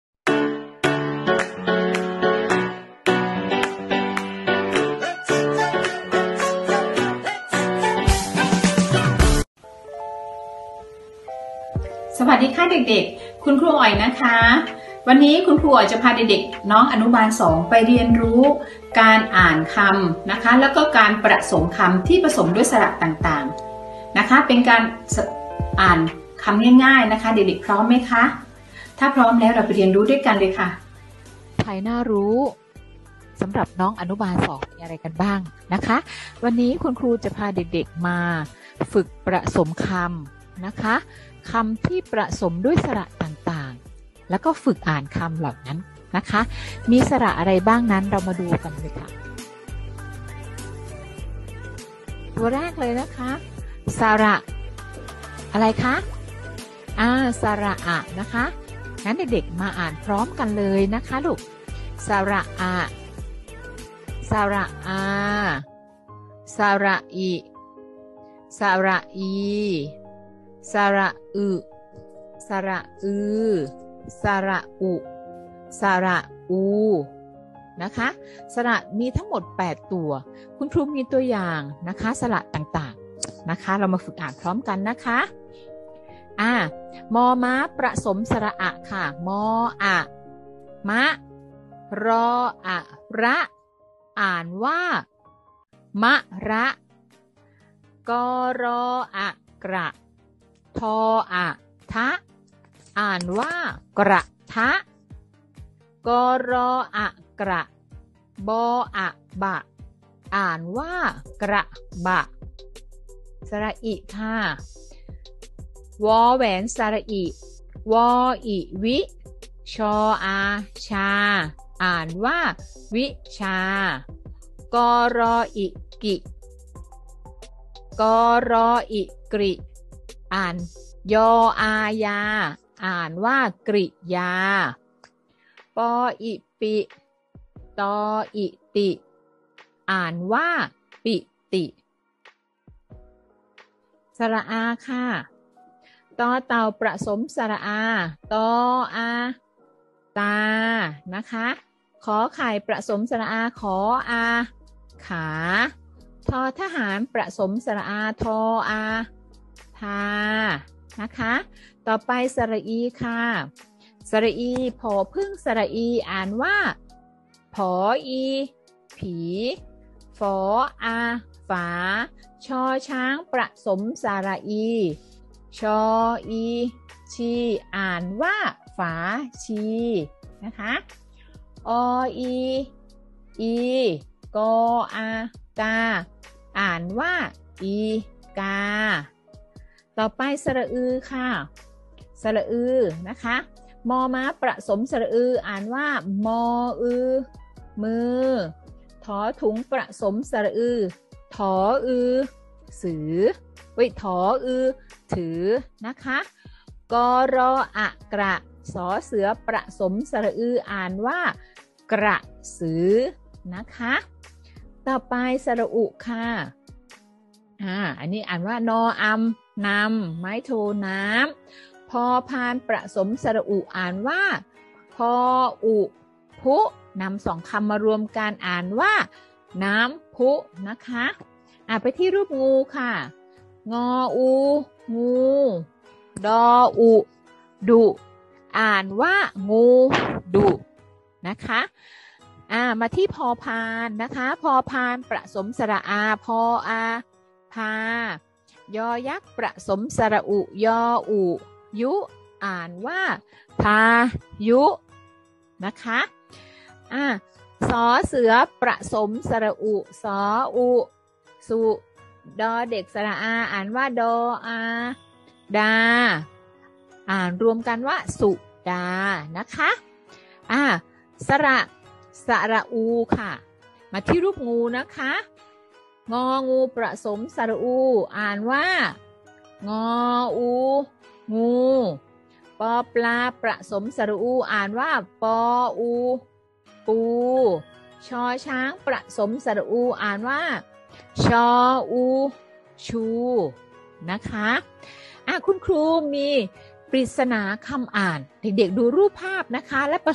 สื่อการเรียนการสอนระดับชั้นอนุบาล 2 เรื่องภาษาไทยน่ารู้
ภาษาไทย เป็นภาษาที่สละสลวย  เรามาฝึกอ่านคำที่ประสมสระกันค่ะและ มีสระอะไรบ้างนั้น น้องๆอนุบาล  2 มาเรียนรู้ไปด้วยกันคะ